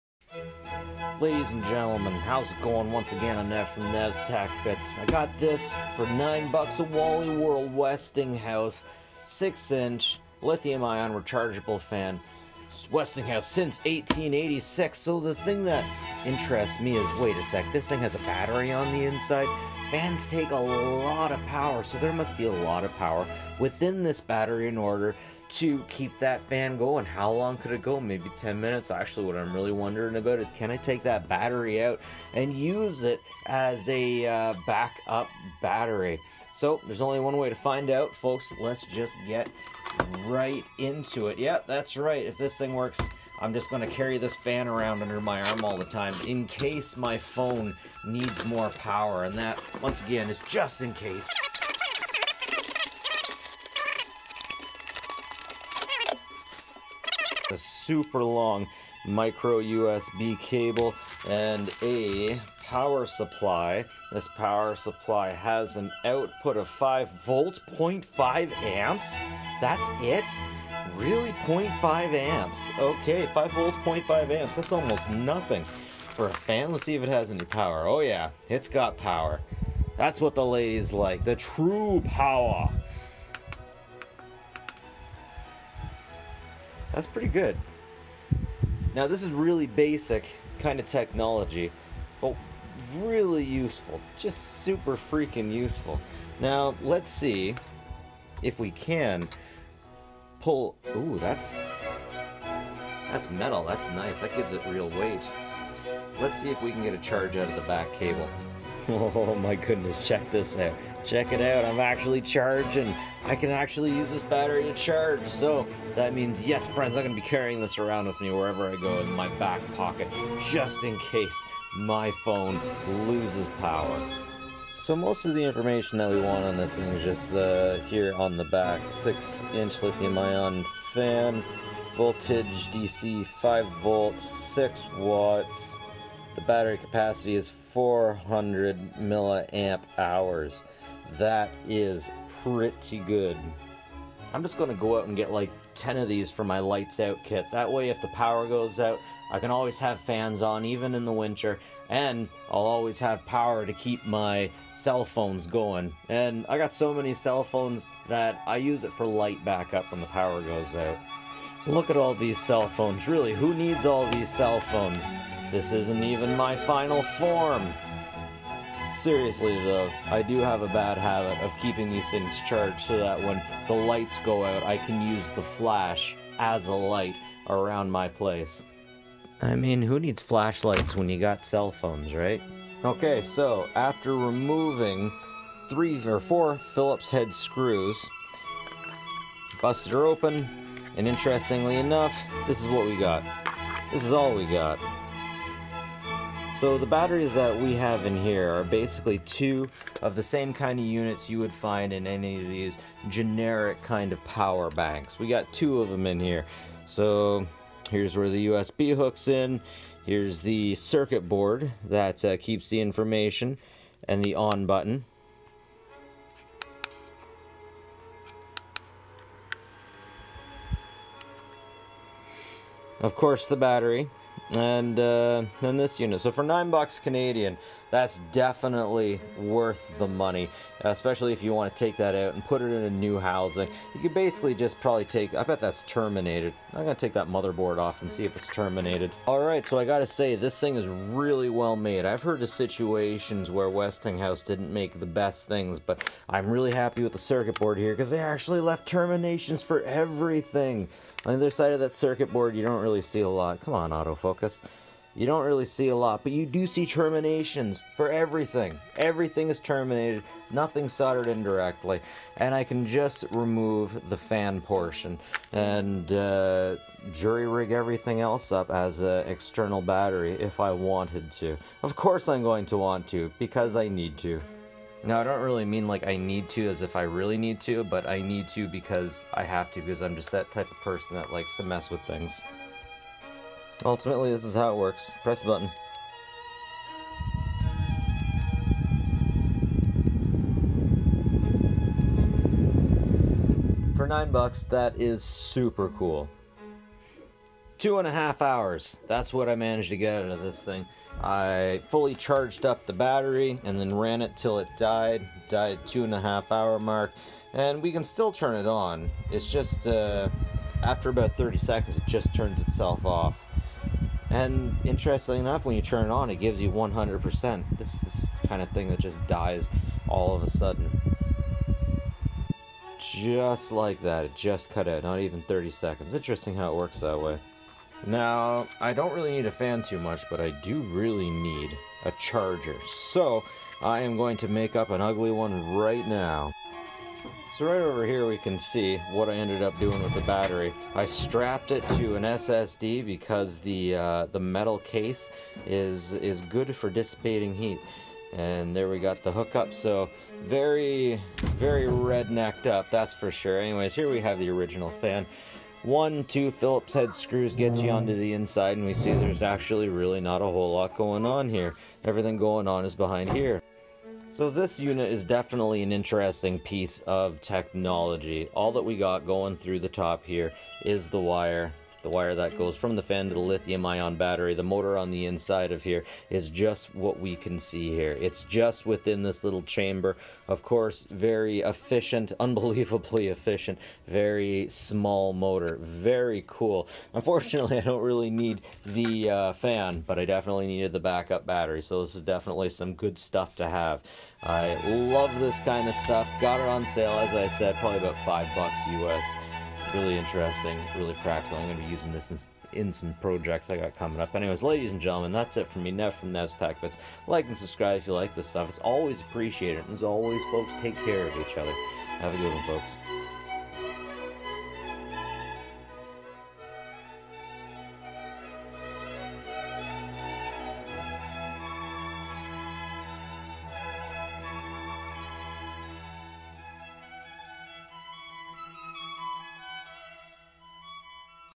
Westinghouse 6" LI-ION Rechargeable Fan Teardown For The Internal Battery & Review
Westinghouse 6" LI-ION Rechargeable Fan Teardown For The Internal Battery & Review. Let's see what is inside

This is an interesting little fan. Seven bucks at Walmart. This unit has two cells on the inside. Almost the same type as e-cigarettes. In this video I tear down the fan to get to the battery that I can use elsewhere.

WSRF807WH -White   WSRF807BK – Black
6” (15CM) Rechargeable Fan,  4 fan speeds.
2M USB Cable and Power Adapter Included
4000 mAh Built In Lithium Battery
More than 9 hours operation in low mode after full charge.